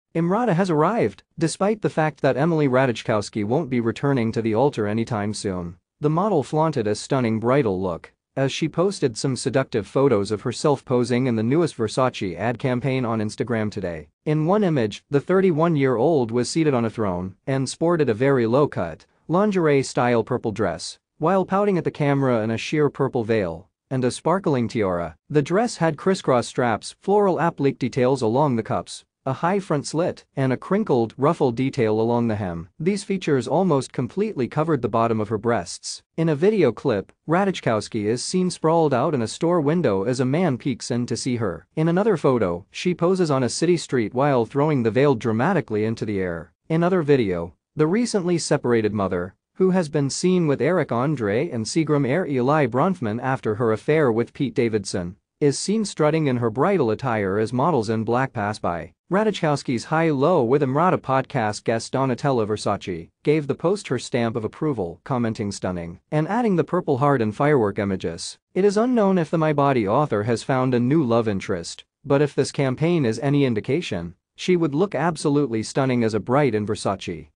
For a Versace advertisement, newly single Emily Ratajkowski portrays a bride wearing a veil.
EmRata has arrived.
Despite the fact that Emily Ratajkowski won't be returning to the altar any time soon, the model flaunted a stunning bridal look as she posted some seductive photos of herself posing in the newest Versace ad campaign on Instagram today.